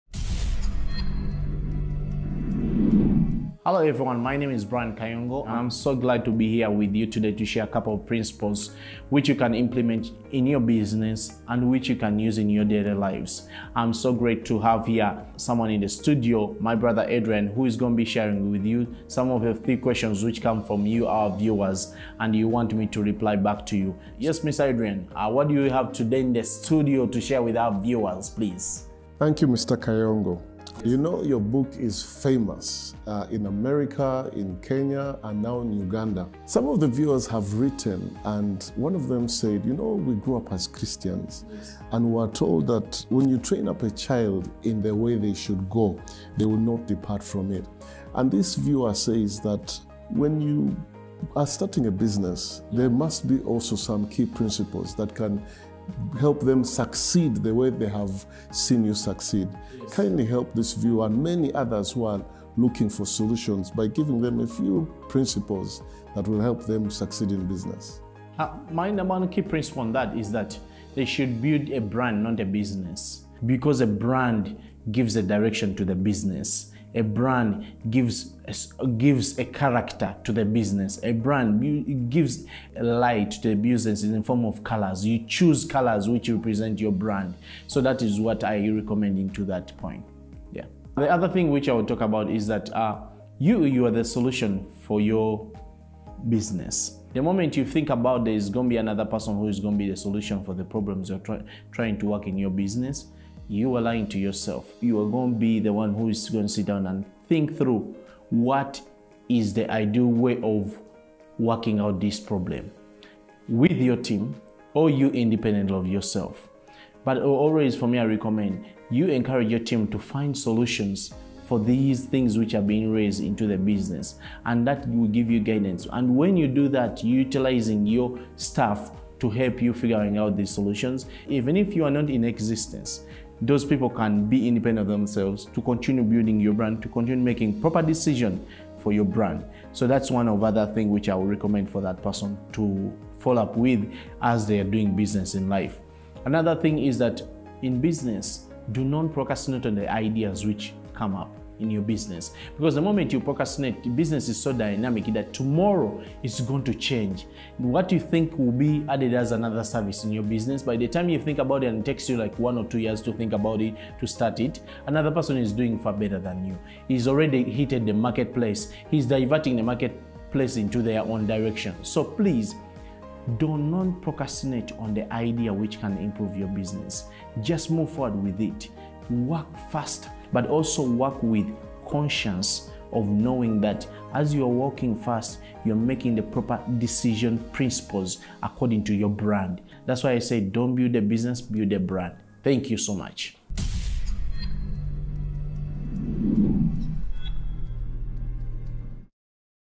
THE THREE PRINCIPALS TO CONSIDER WHEN STARTING A BUSINESS
one of my viewers after  watching one my videos on this channel and reading my book ''the enterprising entrepreneur''  wrote and ask what have i done to make sure that i become successful.
in the video will be sharing with you some few principals that everyone who is starting a business should follow